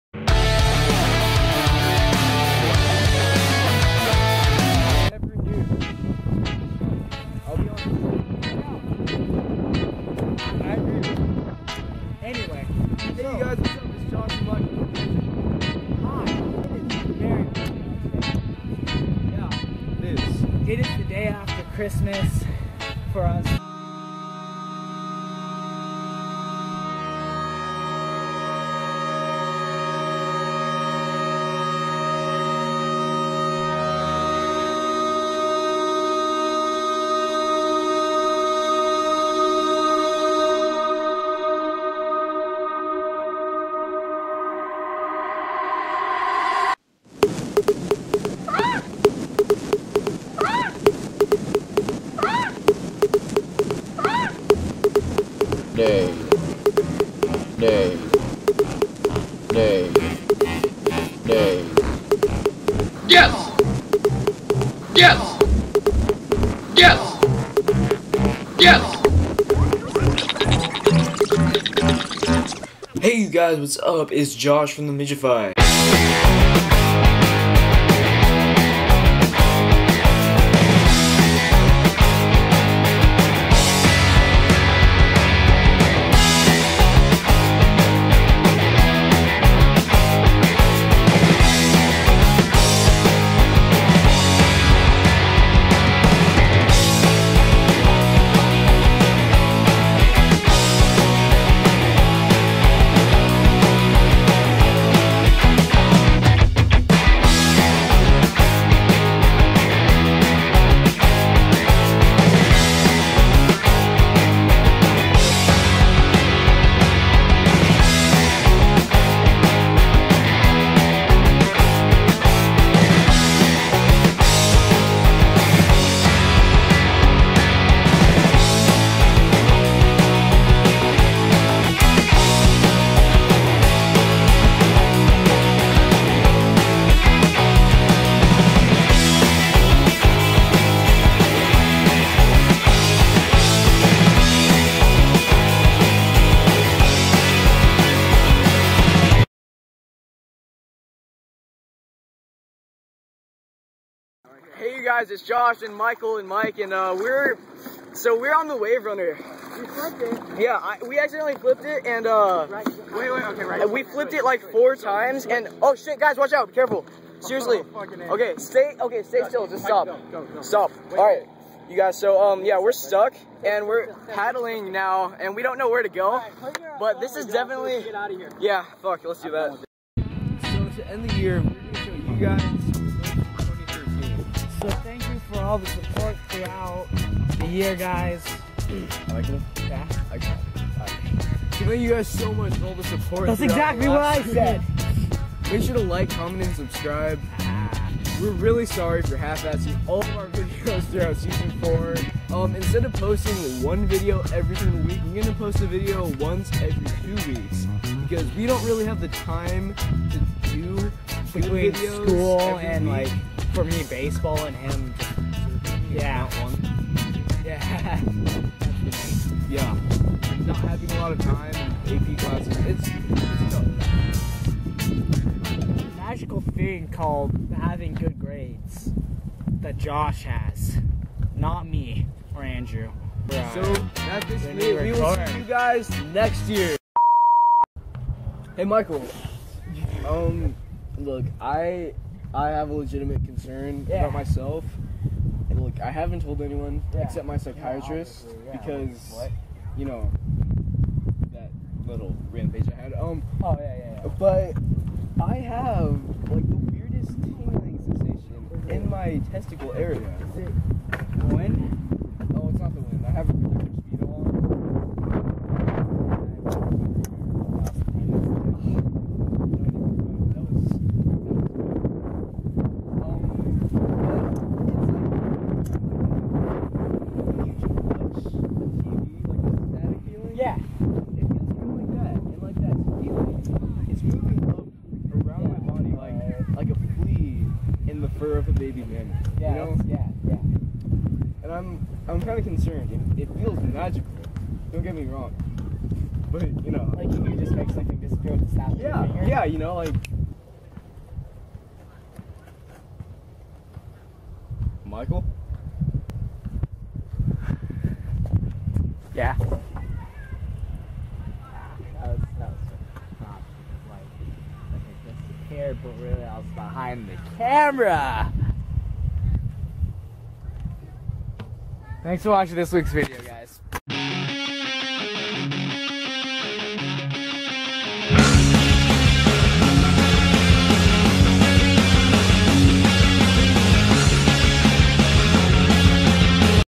TheMijify's 2013 Montage
This is a montage of our year! Sorryy about the wind during our vlog.